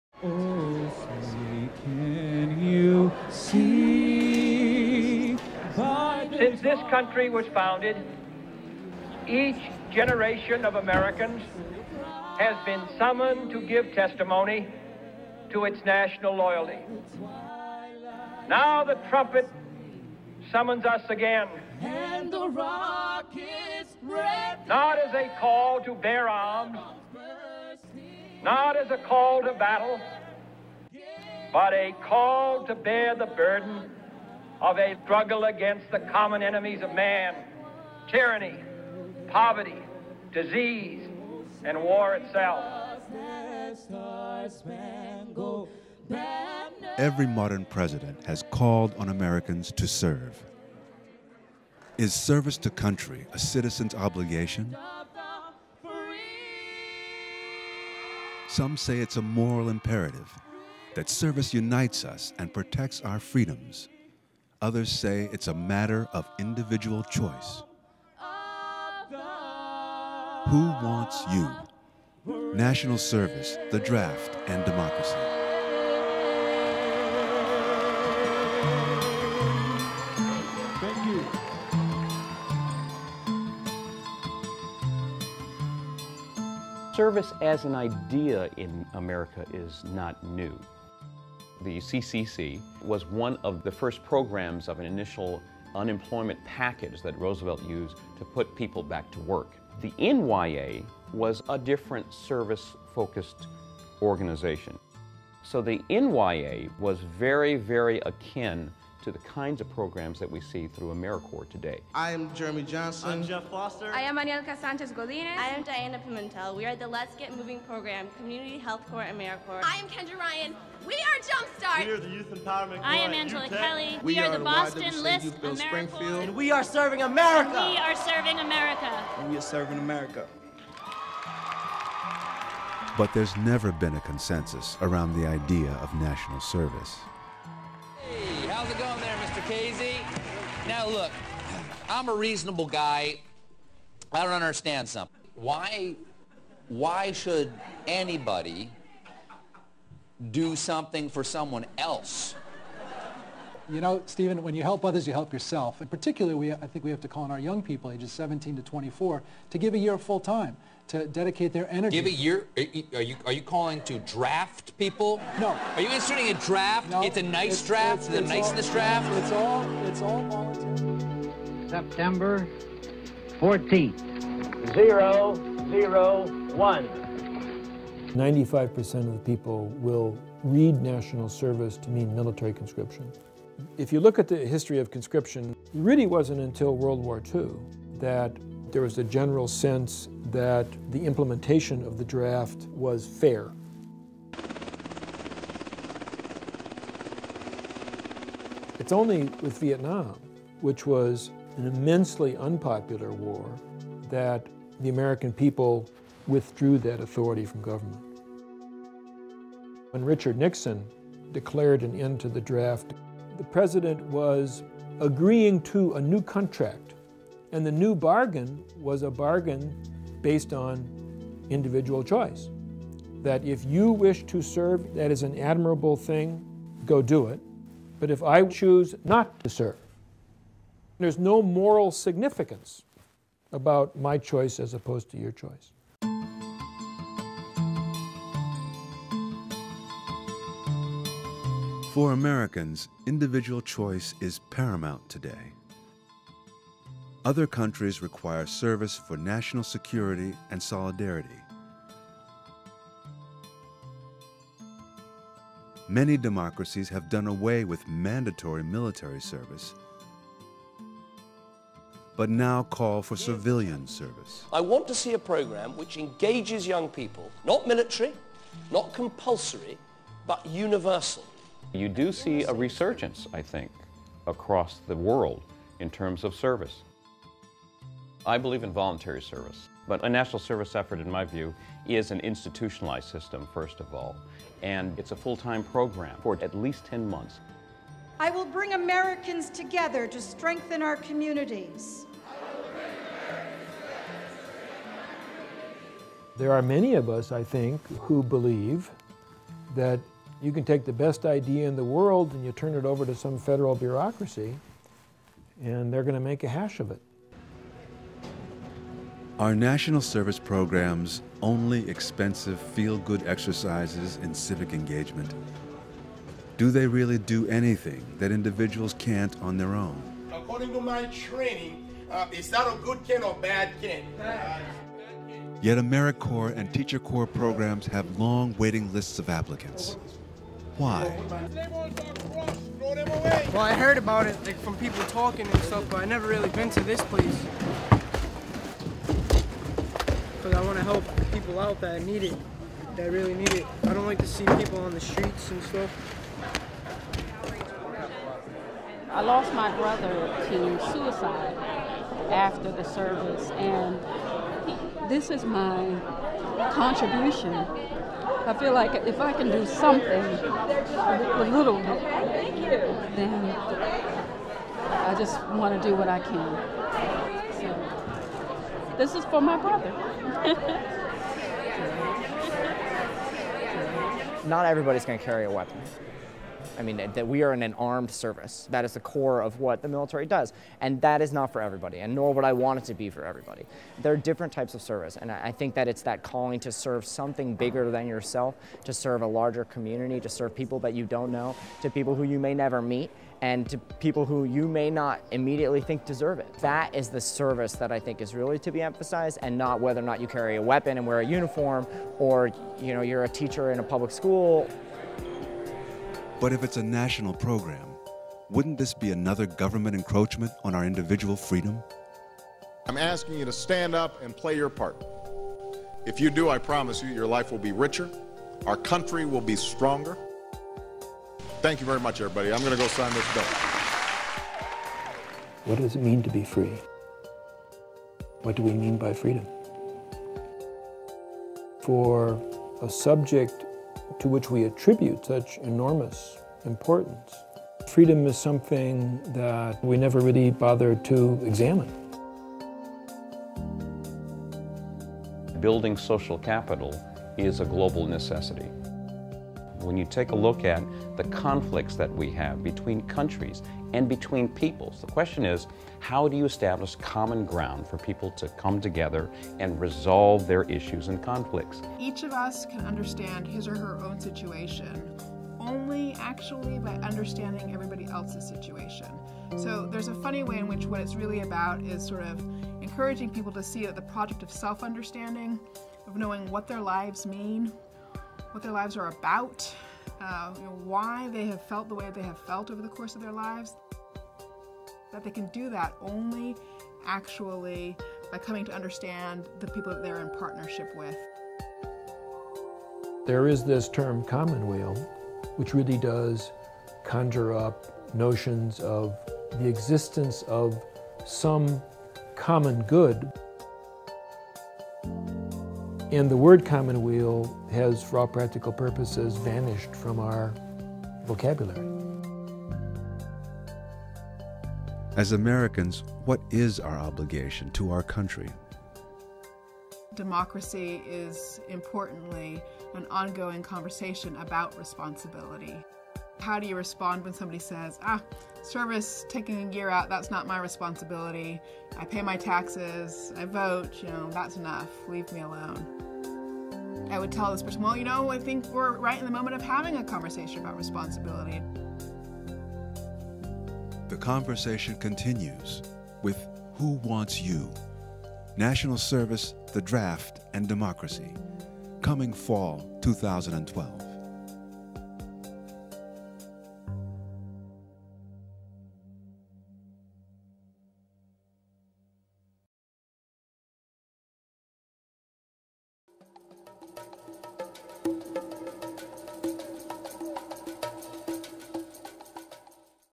WHO WANTS YOU? NATIONAL SERVICE AND DEMOCRACY
Who Wants You? National Service and Democracy challenges us to reflect on what kinds of obligation we have as citizens to serve our country -- whether as civilians or as soldiers. We will see portraits of service today -- from AmeriCorps to the military. We will see portraits of service through our history, including those mandated by the military drafts. And we will hear from those who question any government intervention funding a mandate for a citizen to dedicate time to serve his or her country. The project will energize a national conversation about service, political and social engagement and democracy.